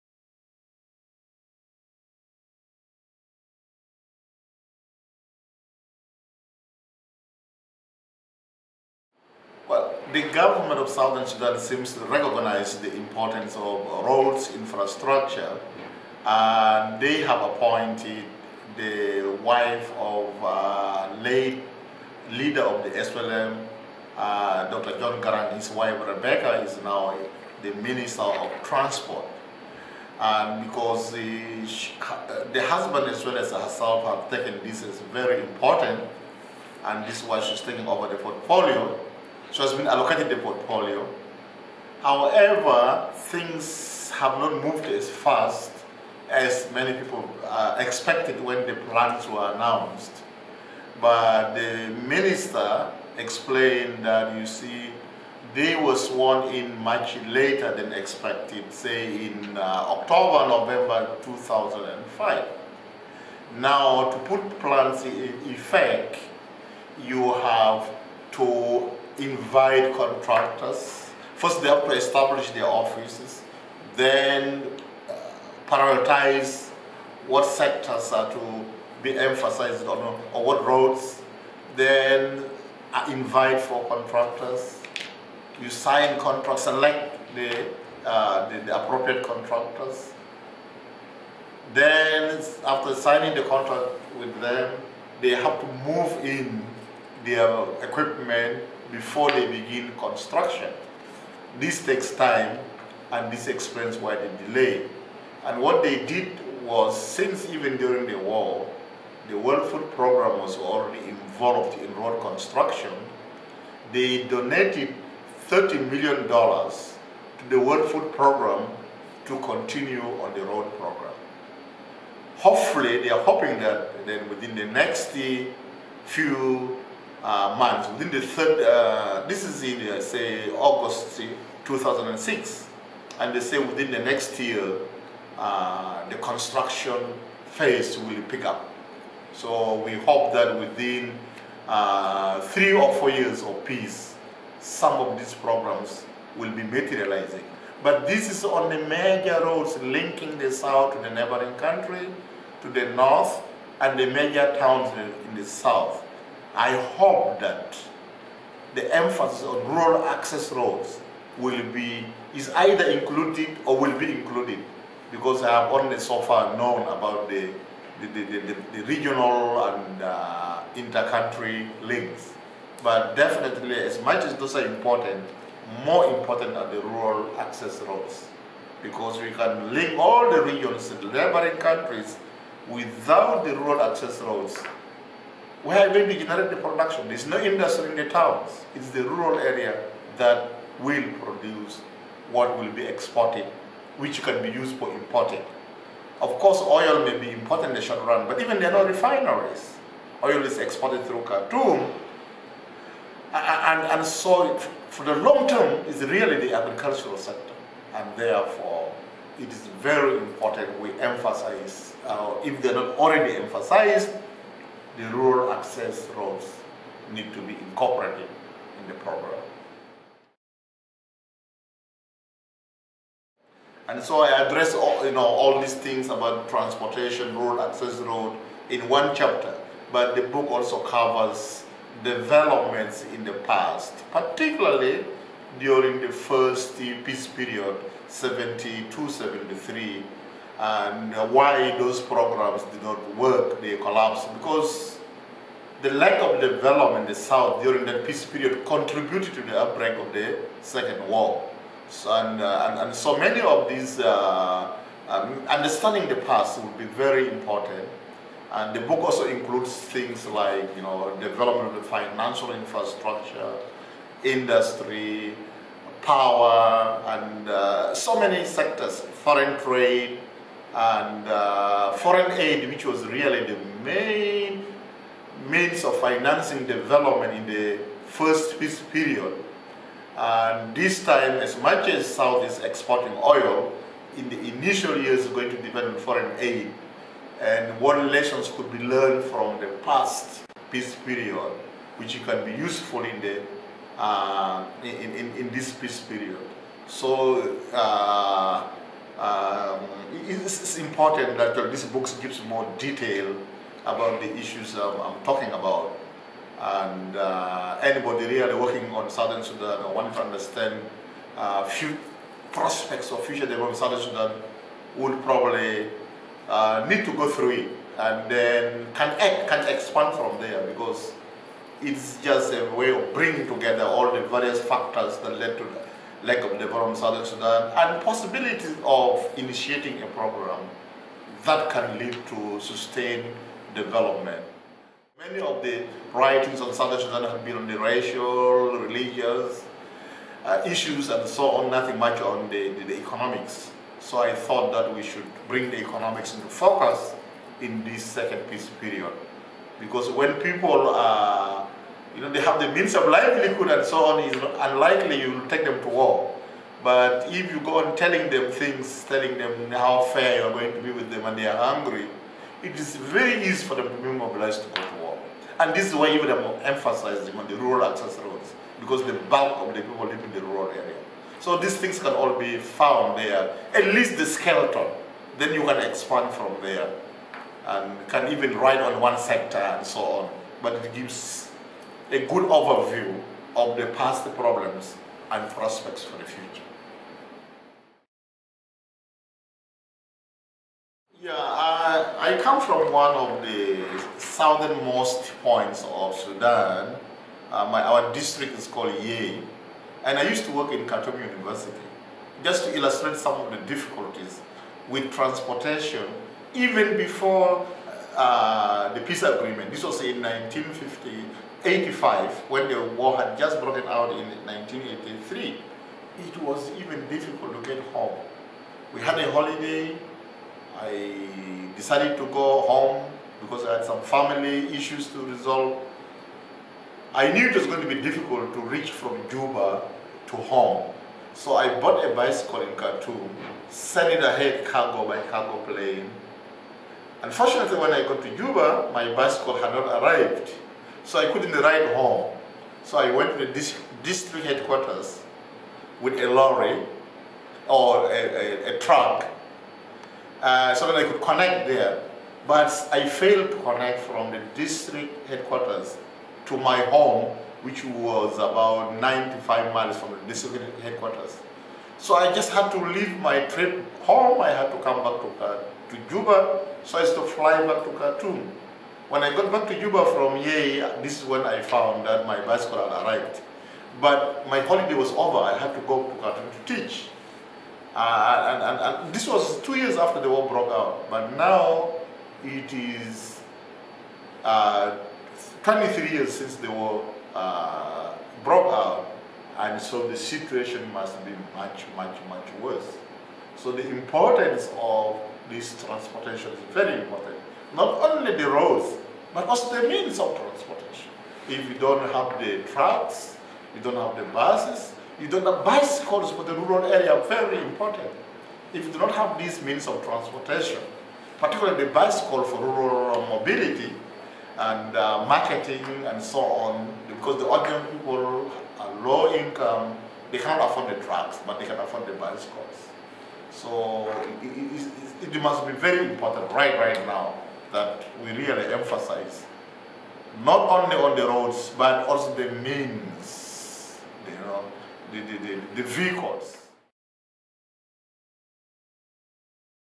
Economic Development in Southern Sudan
Prof. Benaiah Yongo Bure speaks on economic development in southern Sudan, which an emphasis on investment in the transport sector.